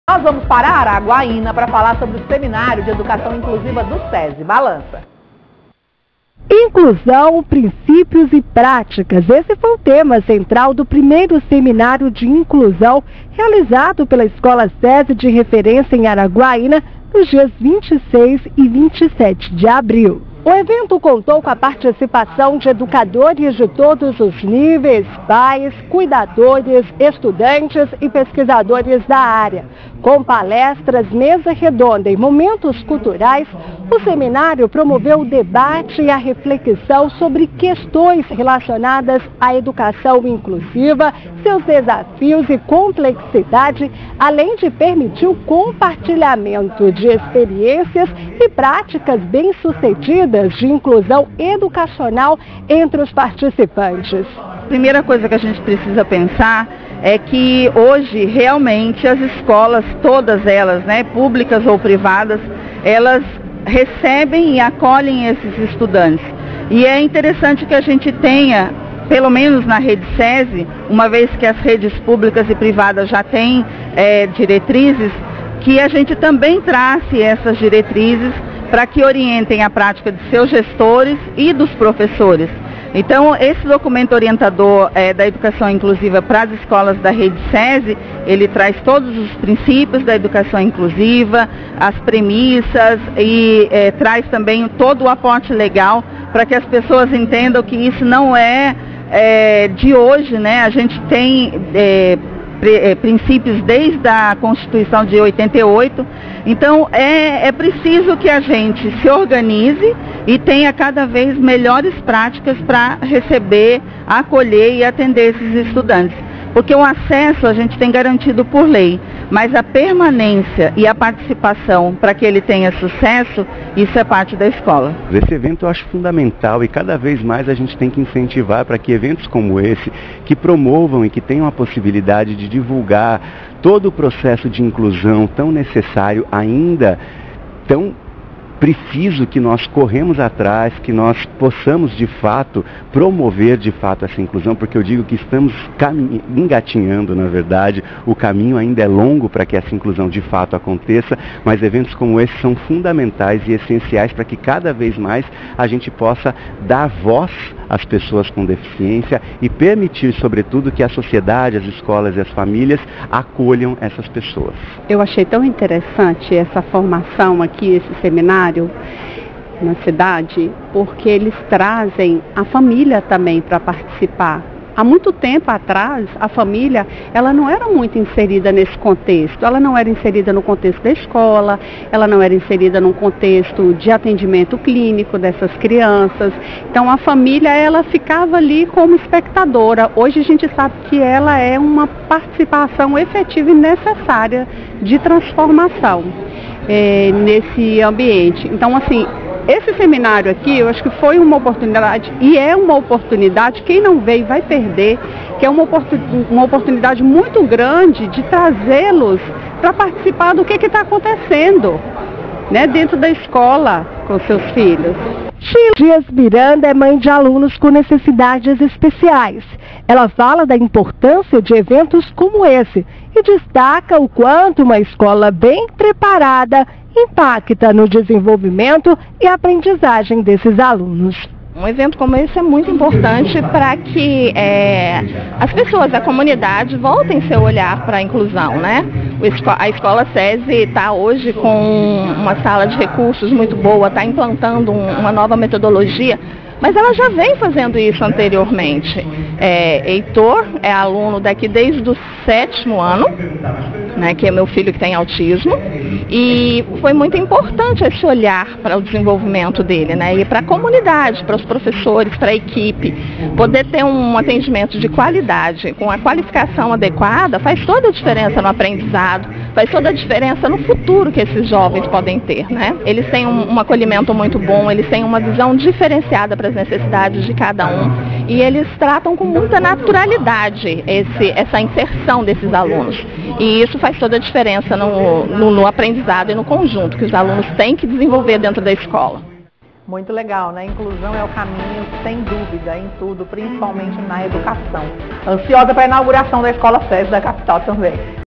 S𝐞𝐦𝐢𝐧𝐚́𝐫𝐢𝐨 𝐝𝐞 𝐄𝐝𝐮𝐜𝐚𝐜̧𝐚̃𝐨 𝐈𝐧𝐜𝐥𝐮𝐬𝐢𝐯𝐚 𝐏𝐫𝐢𝐧𝐜𝐢́𝐩𝐢𝐨𝐬 𝐞 𝐏𝐫𝐚́𝐭𝐢𝐜𝐚𝐬
Inclusão: Princípios e Práticas” é o tema central do primeiro Seminário de Inclusão do Serviço Social da Indústria (SESI) no Tocantins.